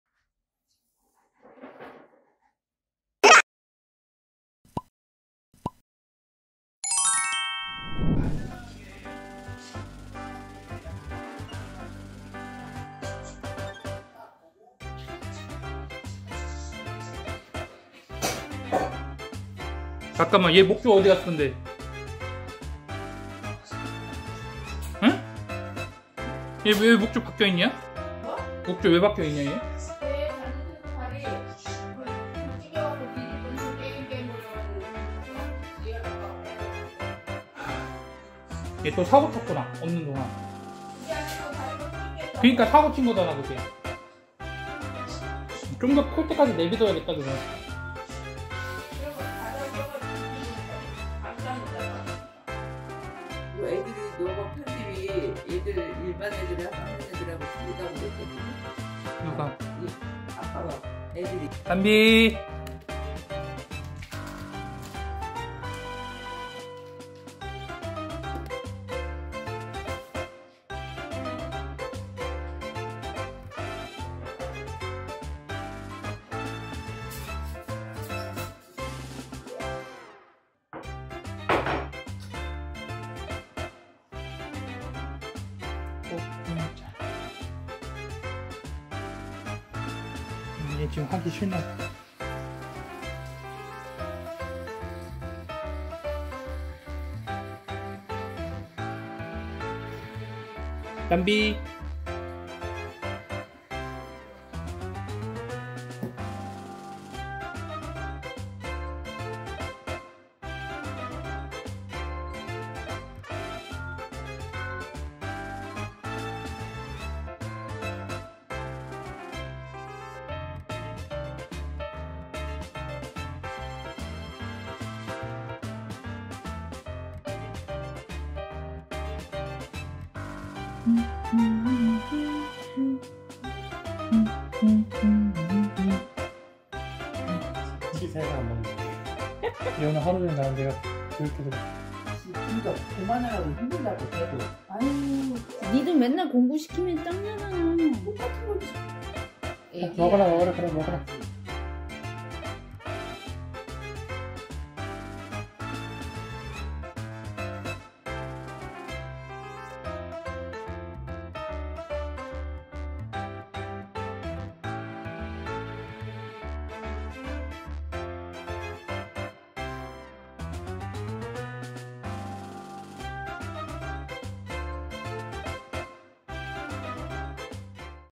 훈련하기 싫어 땡깡 피우는 강아지 아빠에게 대든다!
단비의 하루를 담는 공간
귀여운 단비의 일상을 같이 지켜보아요
단비(3개월령) 2022년 3월 15일 女